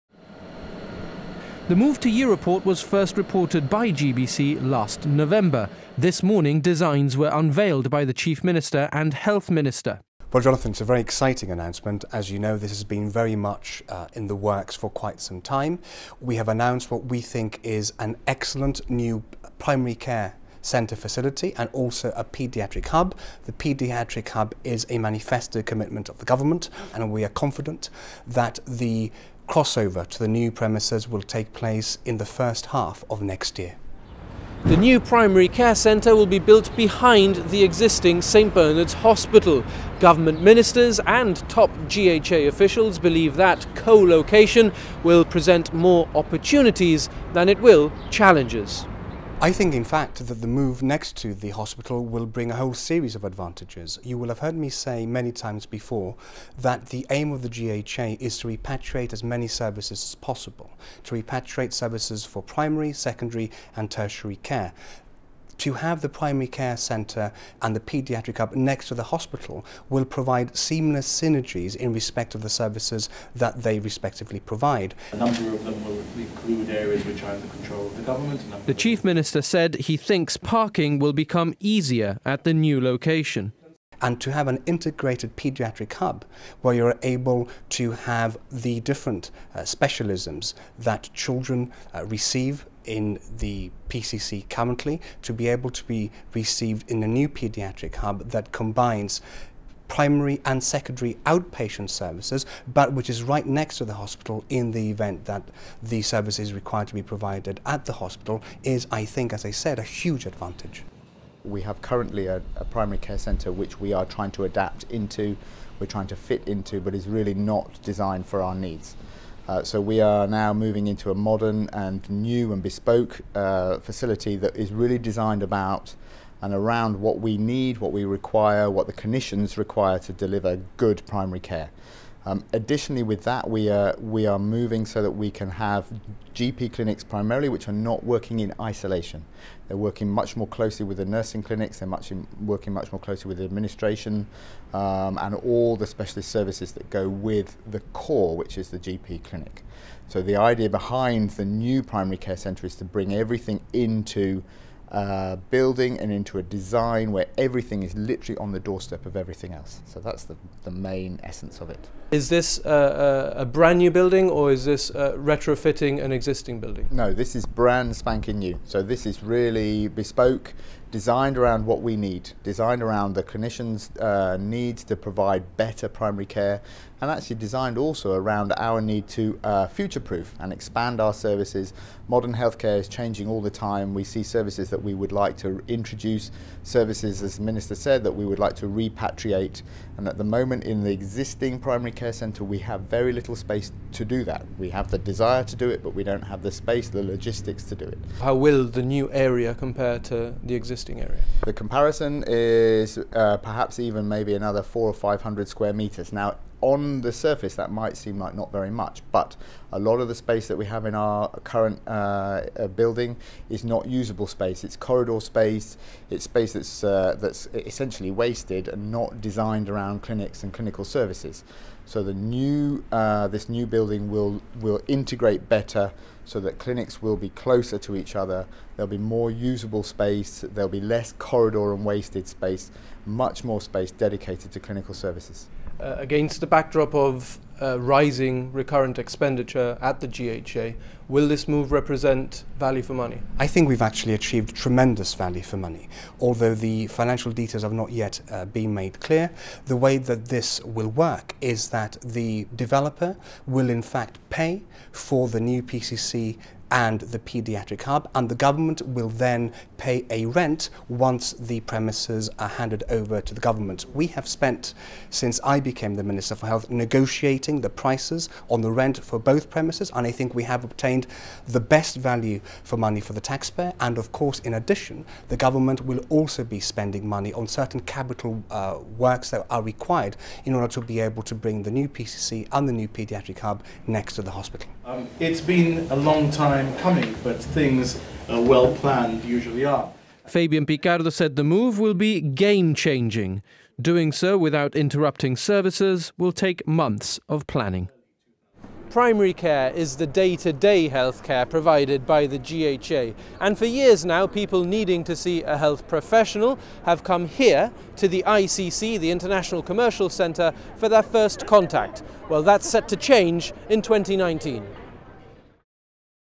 Primary Care Centre to move next to St Bernard's in 2019
The Primary Care Centre will be moving away from the ICC in 2019 and into a purpose-built facility behind St Bernard’s Hospital.

Health Minister Neil Costa said the move would allow the different health services provided by the Gibraltar Health Authority (GHA) to be more integrated, taking the overall level of service "from very good to excellent".

Primary Care is the day-to-day healthcare provided by the GHA and for years now people needing to see a medical professional have gone to the International Commercial Centre (ICC) for their first contact. That is set to change "in the first half of 2019".

The new Primary Care Centre will be a new three-storey building built behind St Bernard’s Hospital. Government Ministers and top GHA professionals think the co-location will provide more opportunities than it will challenges.

Top GHA practitioners said the new centre will be specifically designed and built with the service in mind. A Paediatric Hub will be created to improve the service provided to children.

The move to Europort was first reported by GBC in November; on Monday morning designs were unveiled by the Chief Minister and the Health Minister. Fabian Picardo said it would be a "giant leap forward".